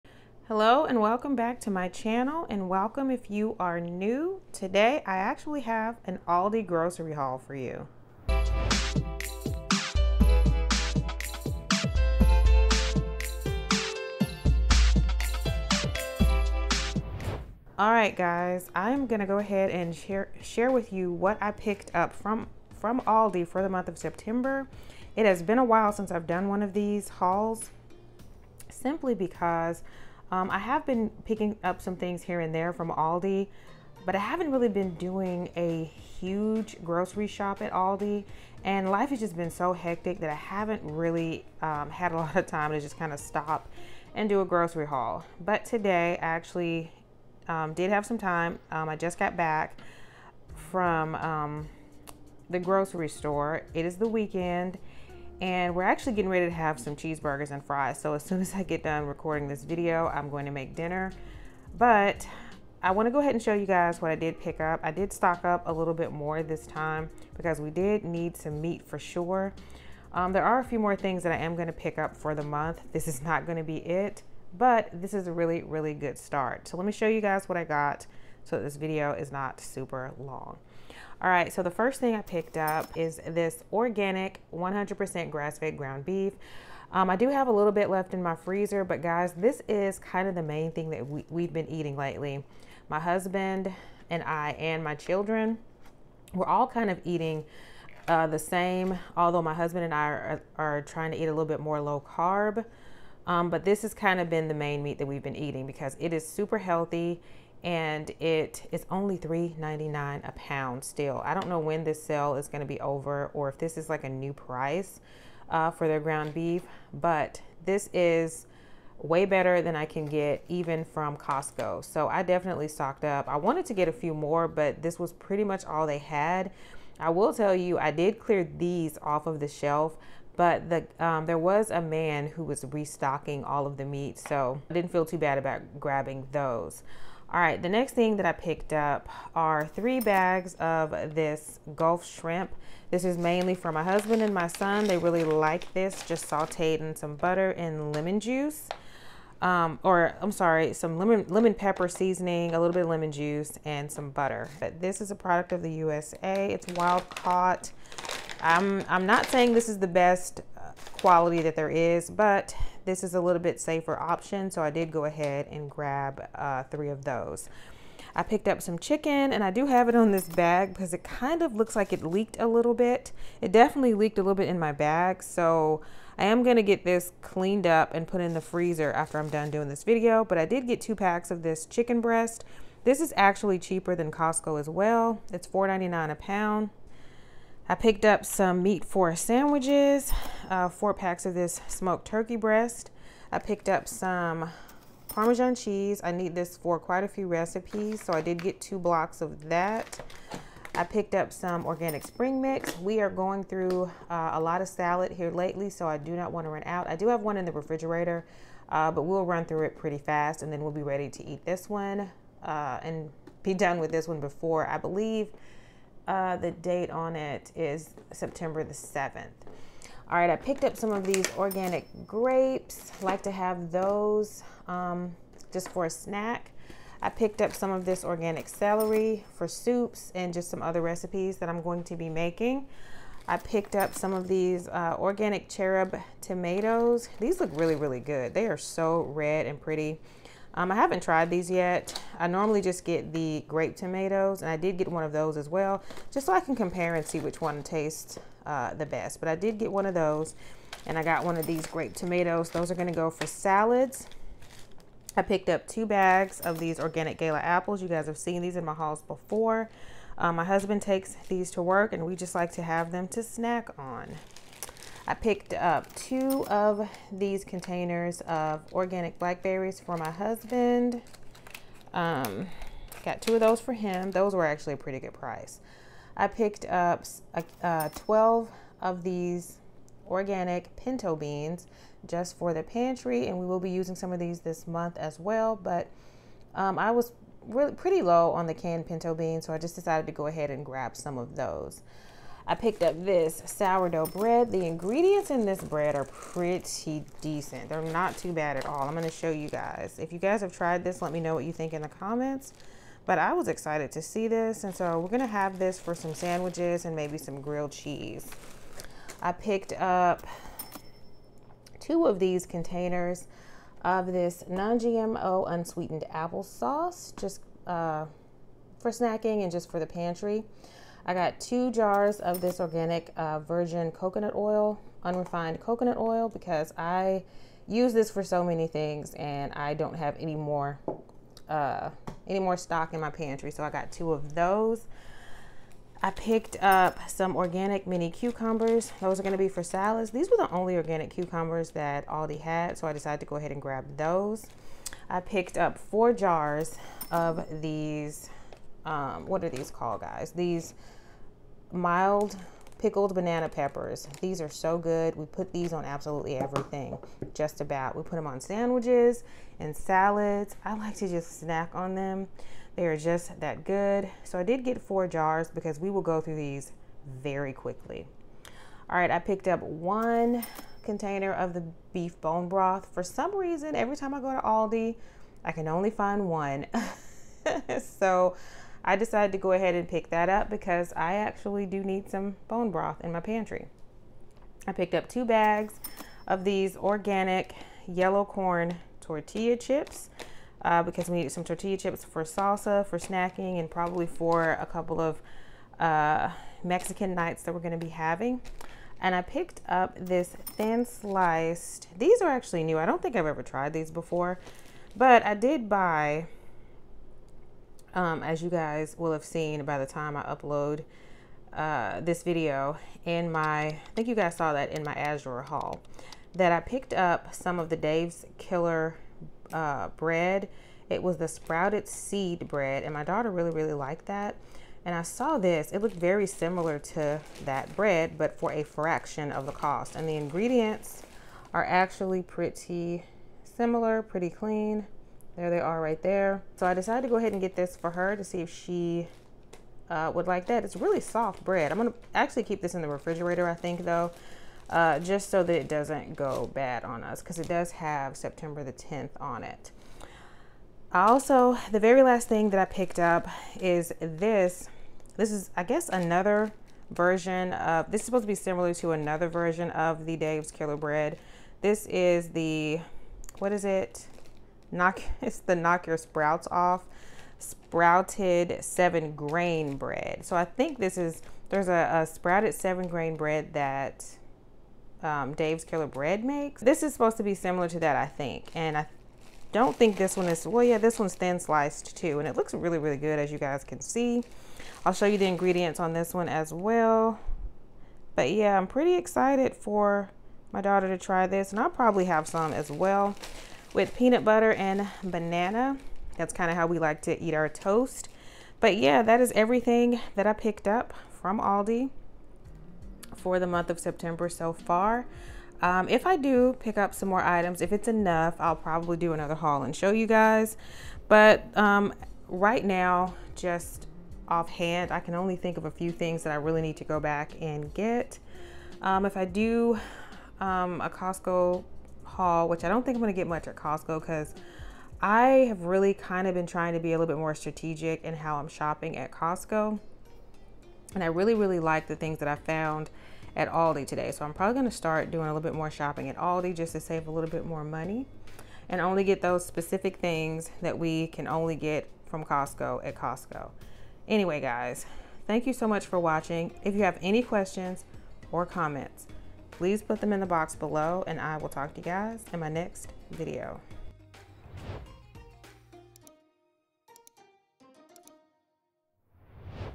Aldi Grocery Haul with PRICES // September 2024
Welcome, and welcome back, if you are new! Sharing the grocery items that I picked up from Aldi for the month of September, 2024. Some items are new and some are staples. PRICES are INCLUDED for each item as usual.

aqizqz

SHOPKICK CODE
YAY332422

5X7ZRF

NF4RU